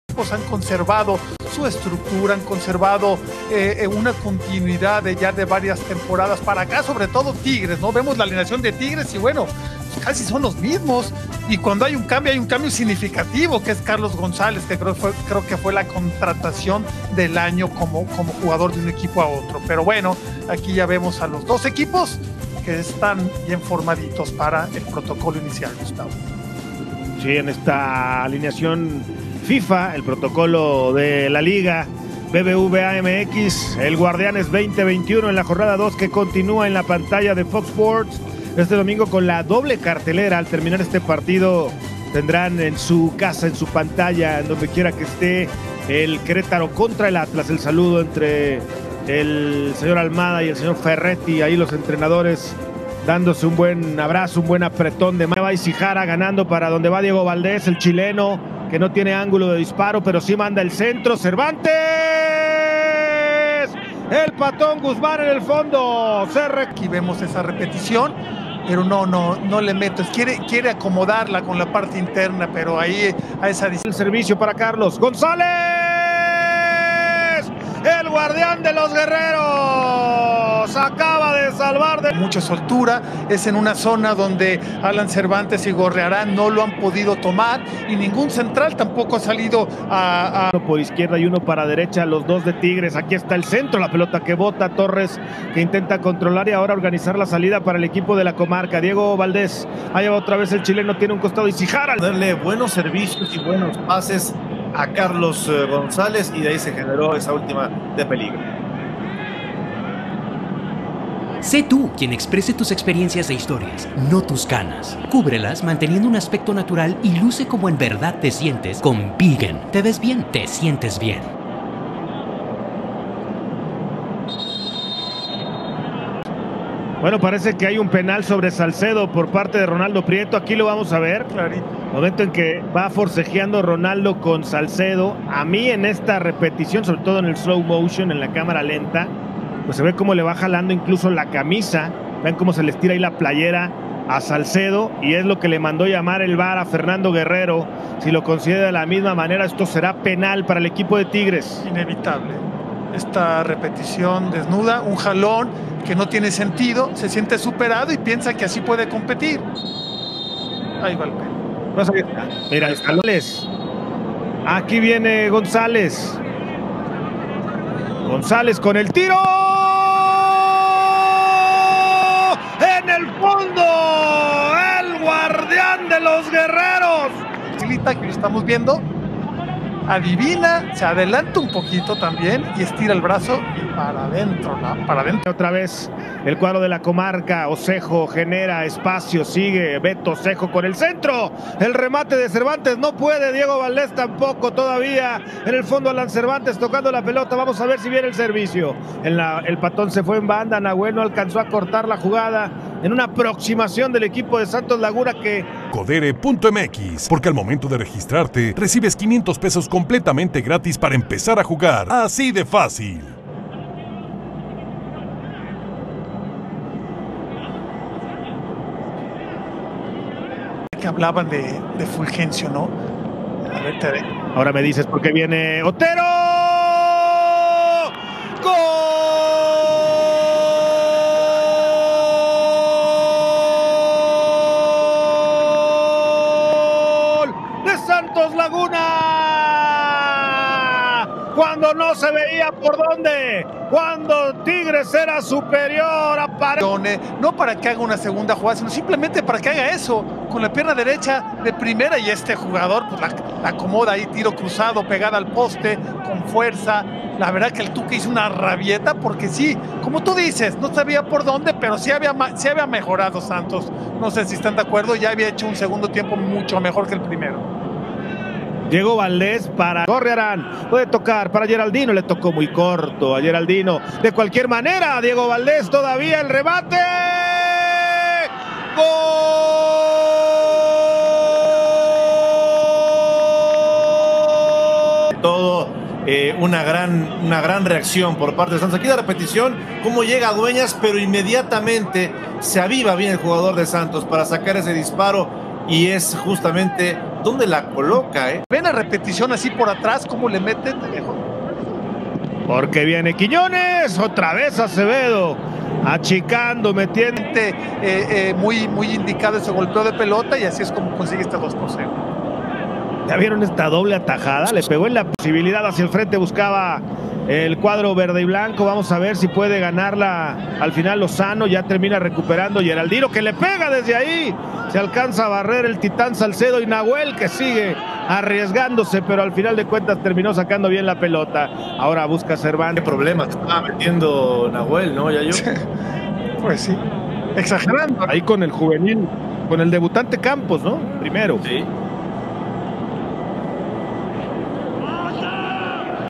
Resumen y Goles | Santos vs Tigres | Liga BBVA MX - Guard1anes 2021 - Jornada 2
Club Santos 2-0 Tigres
Domingo, 17 de enero del 2021
19:00 horas
Estadio Corona
Árbitro: Fernando Guerrero

El partido

Club Santos derrotó dos goles por cero a Tigres en el partido correspondiente a la Jornada 2 del Torneo Guard1anes Clausura 2021.

Goles:

Minuto 74: Tras una gran jugada que crearon entre los laguneros, Juan Otero, recibió una asistencia de Diego Valdés, y no perdonó a Nahuel Guzmán para marcar su primer gol en México.

Minuto 79: Eduardo Aguirre entró de cambio y fue fundamental. Tras un disparo de Ignacio Jeraldino que rechazó Guzmán, ‘El Mudo’ estuvo atento para concretar en el marco felino para marcar el segundo tanto del partido.

JUGADOR DEL PARTIDO

Carlos Acevedo sigue siendo el hombre clave en Club Santos. Hoy lo volvió a demostrar.

Por ello, el Centro de Innovación Tecnológica nos muestra las estadísticas del cancerbero lagunero.

EL DEBUT

Omar Antonio Campos entró en la recta final del partido para realizar su debut en el Máximo Circuito. El futbolista nacido en la Ciudad de México tiene formación en la LIGA BBVA MX desde la categoría Sub-13, todos con Santos Laguna.  Hoy, el elemento de 18 años logró jugar por primera vez con el primer equipo.

La LIGA BBVA MX felicita a Omar Antonio y sigue buscando ser una cuna de grandes estrellas para el futuro.